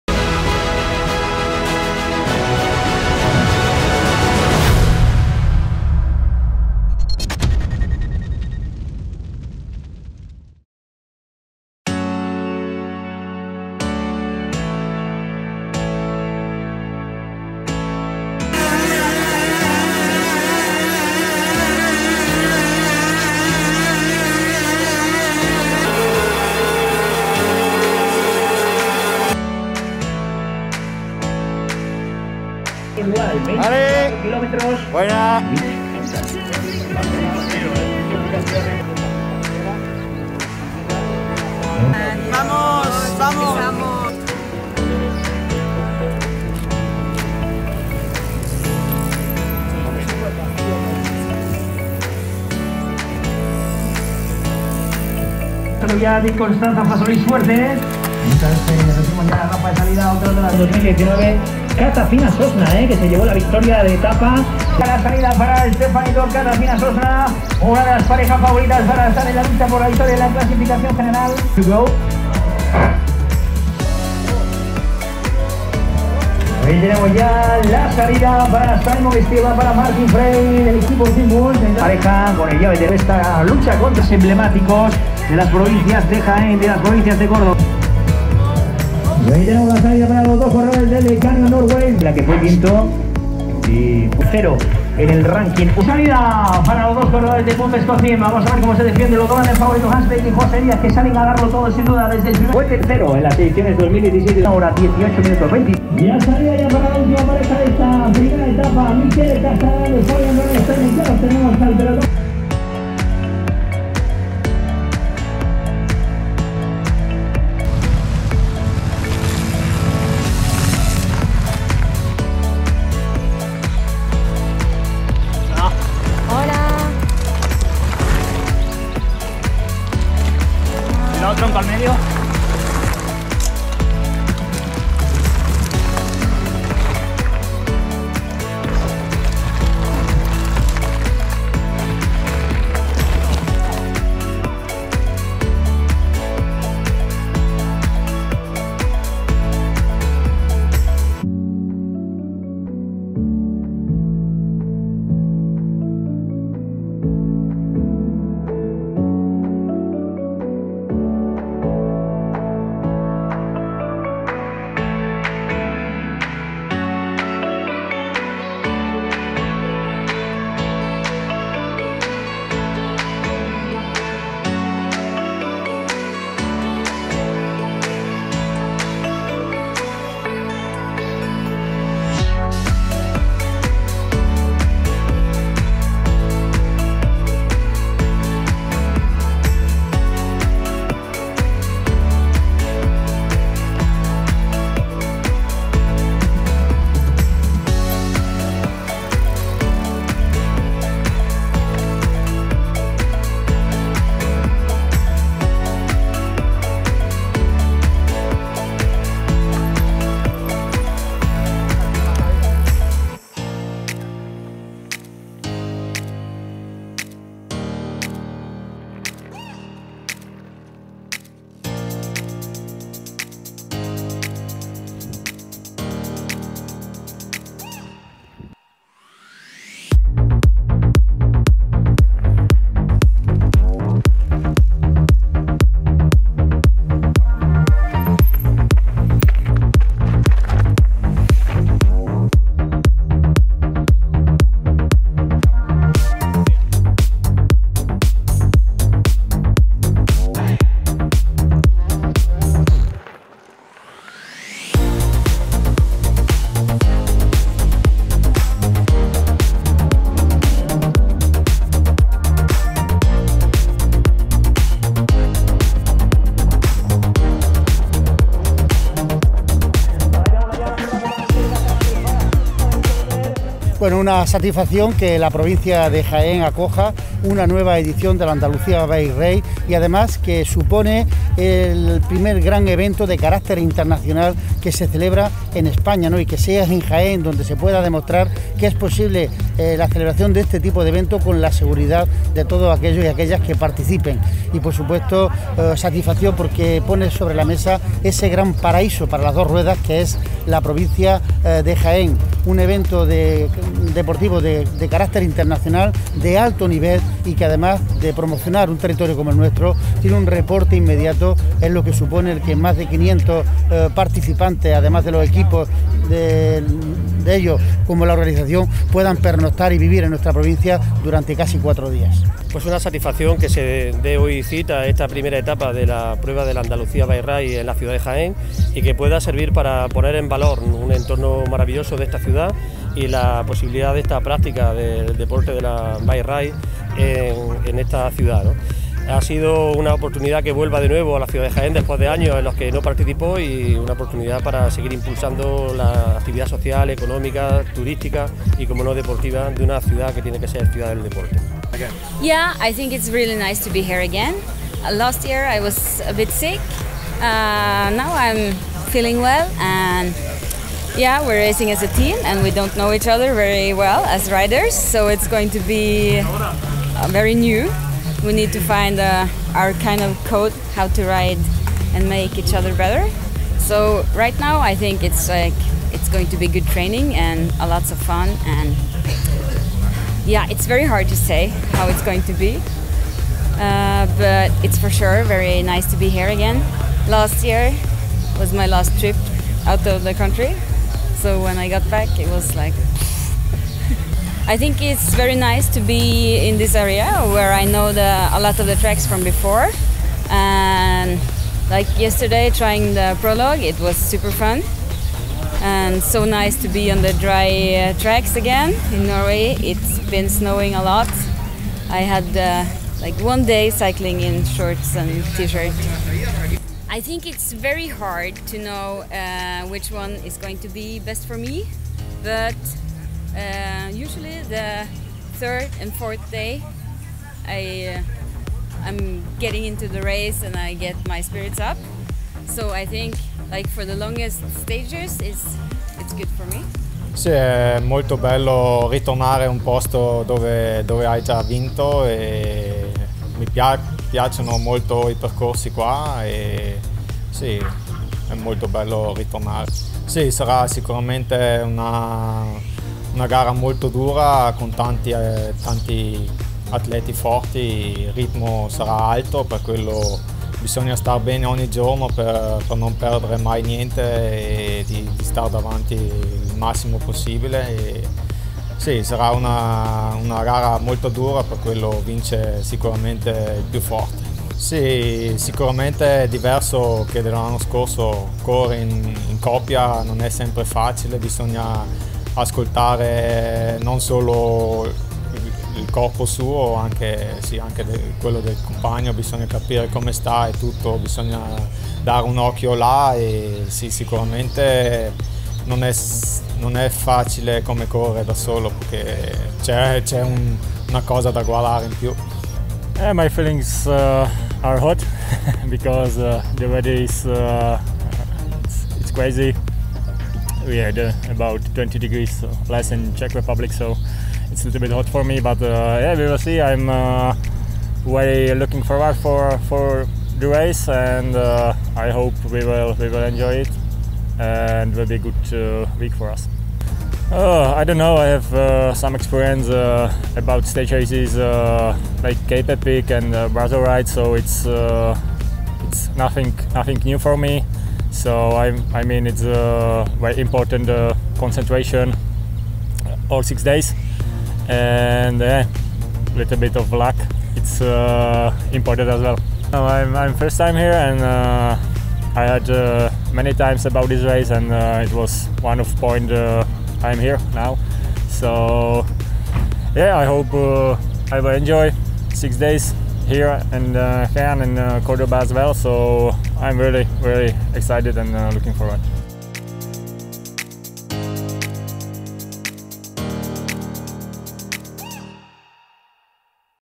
Andalucía Bike Race 2021 Etapa 1
La 1ª etapa de la Andalucía Bike Race 2021 ha sido una prueba a modo de contrarreloj, de poco más de 24 kilómetros y 870 metros de desnivel.

Una jornada marcada por el calor y por el terreno seco, en la que los participantes se enfrentaron a tramos duros como el ascenso al Neveral.

Una jornada con más de 200 salidas por parejas que ha durado desde las 10 de la mañana hasta las 16 horas, donde cada pareja salía con un decalaje de 2 minutos entre ellas.

Clasificaciones FLASH STAGE UCI Marathon Series Men

01:14:02 Urs Huber – Simon Schneller                           TEAM BULLS
01:13:32 Samuele Porro – Daniel Gesimayr                   TREK PIRELLI
01:13:59 Tiago Ferreira – Wout Alleman                        DMT RACING TEAM

 Clasificaciones FLASH STAGE UCI Marathon Series Women

01:37:15 Irina Lutzelschwab – Jennie Stenerhag ABRO-Knippcycling Schär Gärten
01:37:15 Hildegunn Hovdenak – Janine Shcneider       TYSK-NORGE
01:37:45 Katazina Sosna – Stefanie Regina                   TORPADO SUDTIROL